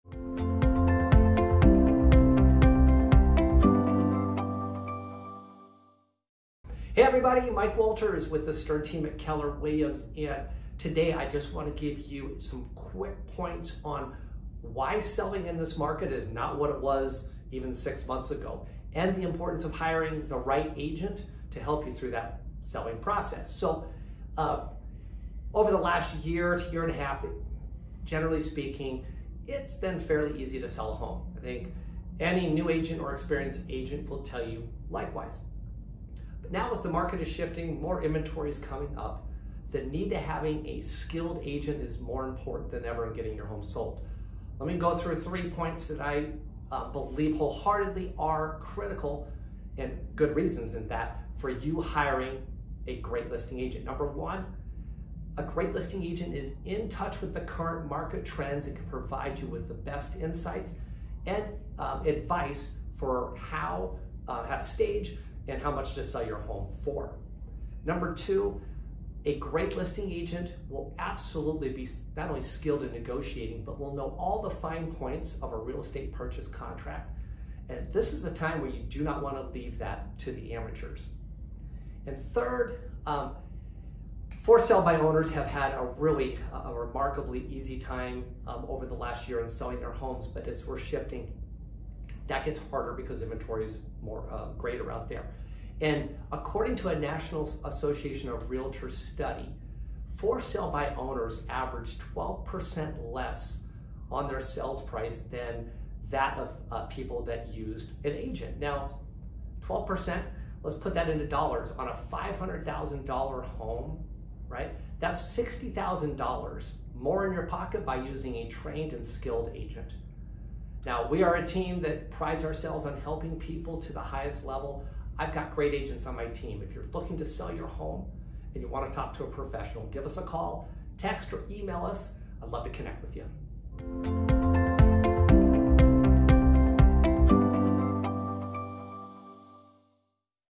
Stern Market Minute: How Selling Your Home Has Changed
Selling a home has changed dramatically in the past few months. Now more than ever, it's vital to have a seasoned agent when looking to sell your home!

In this Stern Market Min, we go over the top three benefits of hiring a realtor when you sell a home!